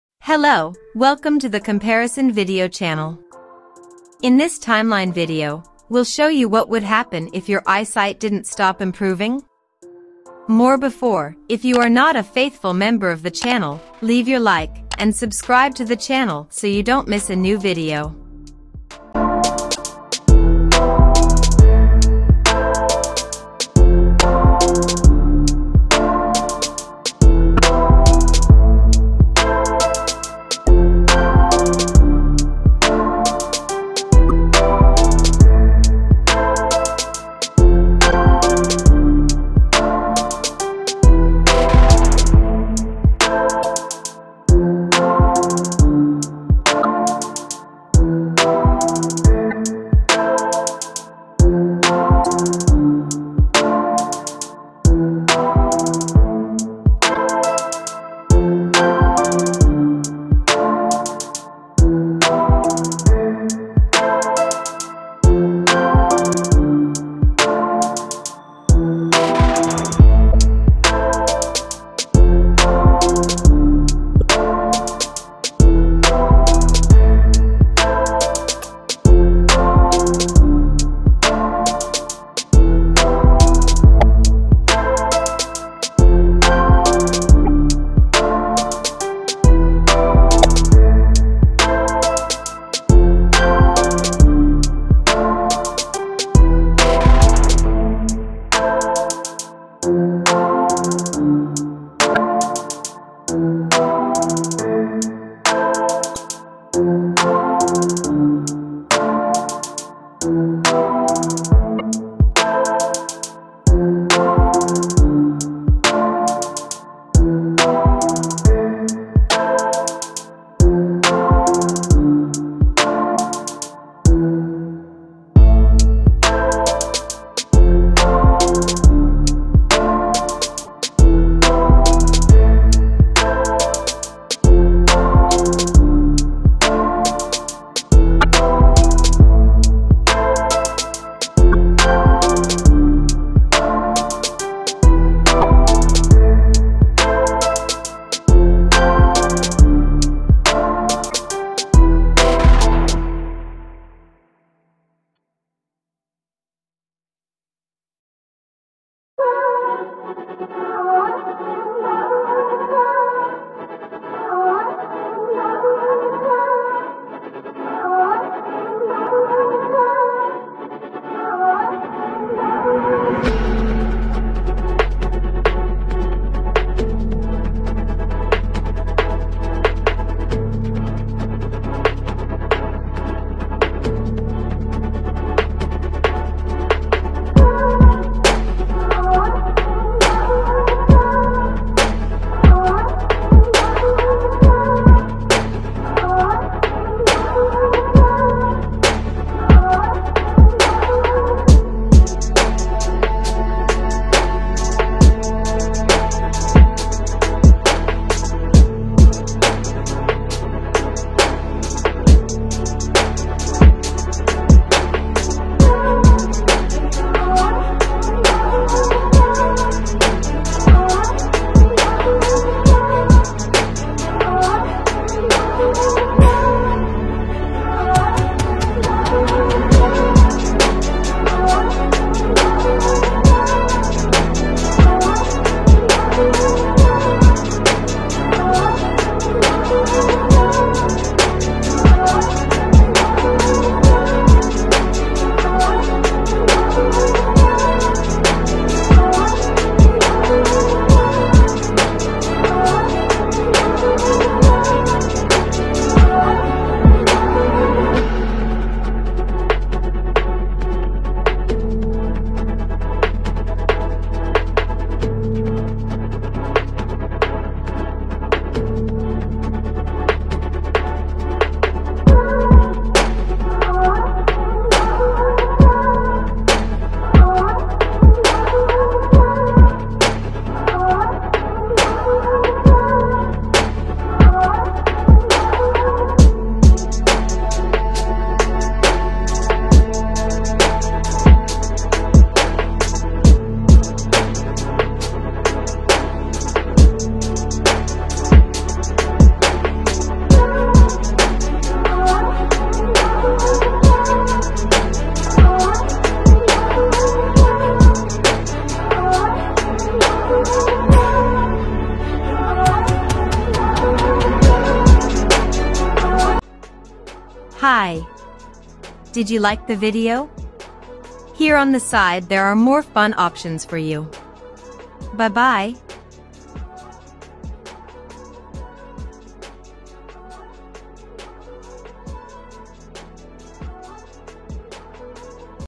What If Your Eyesight Kept Improving? | TIMELINE - COMPARISON VIDEO
In this timeline video, we will show you what would happen if your eyesight kept getting better.

⚠️ This probability comparison/comparison video is based on relevant sources and community discussions.

All numbers and facts listed might not be up to date, valid, or in any specific order.